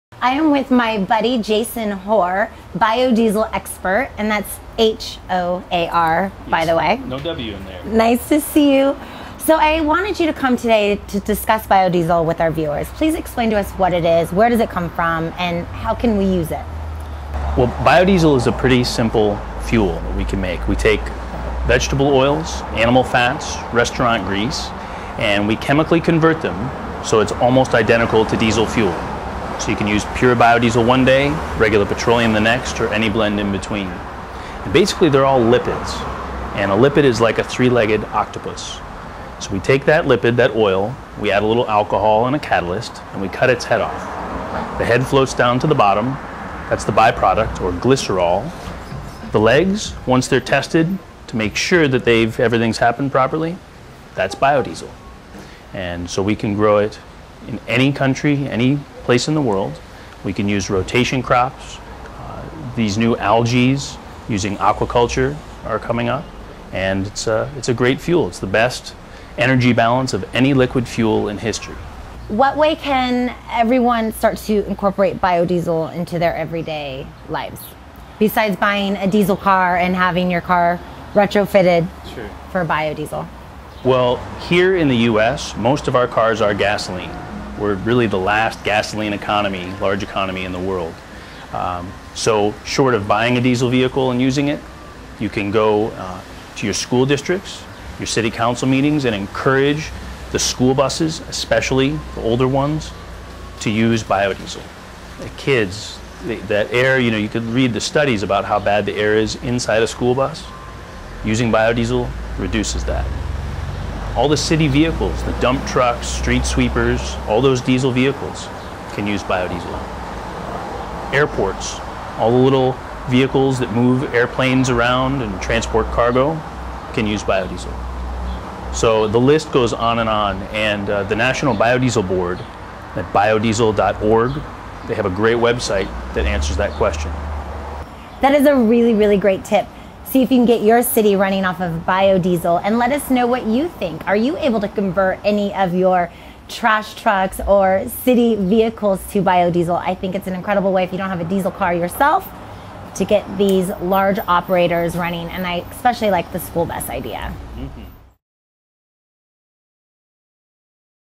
Bio-Diesel for City Gas Guzzlers
Jason Hoar, Bio- Diesel expert explains to Melissa McGinnis from Greenopolis TV that we can all "use" bio-diesel by suggesting to our City Officials to switch to bio- diesel fuel for our city trucks and buses.

Bio-diesel is a pretty simple fuel we can make from vegetable oils, animal fats, and restaurant grease. We chemically convert them so it's almost identical to diesel fuel. So you can use pure bio-diesel one day, regular petroleum the next, or anywhere in between. Basically, they're all lipids, and a lipid is like a three-legged octopus. So we take that lipid, that oil, we add a little alcohol in the catalyst, and we cut its head off. The head floats down to the bottom- that's the by-product, glycerol- and the legs, once they're tested and everything has happened properly- then that's bio-diesel. So we can grow it in any country, any place in the world. We can use rotation crops, these new algaes using aqua-culture are coming-up; and it's a great fuel. It's the best energy-balanced of any liquid fuel in history.

In the U.S., most of our cars are gasoline, we're really the last gasoline economy in the world, so short of buying a diesel vehicle and using it you can go to the school districts, to city council meetings and encourage the school buses, especially older ones, to use bio-diesel. You can read the studies on how bad the air is inside the school bus and using bio-diesel reduces that. All the city vehicles, the dump trucks, the street sweepers, all those diesel vehicles, could use bio-diesel. At airports, the little vehicles that move airplanes around (the transport cargo), can use bio-diesel. So the list goes on and on, and the National Bio-diesel Board, at bio-diesel.org- they have a great website that answers that question.

See if you can get your city running off on bio-diesel and let us know what you think. Are you able to convert any of your trash-trucks or your city vehicles to bio-diesel? It's an incredible way if you don't have a diesel car yourself to get these large operators running.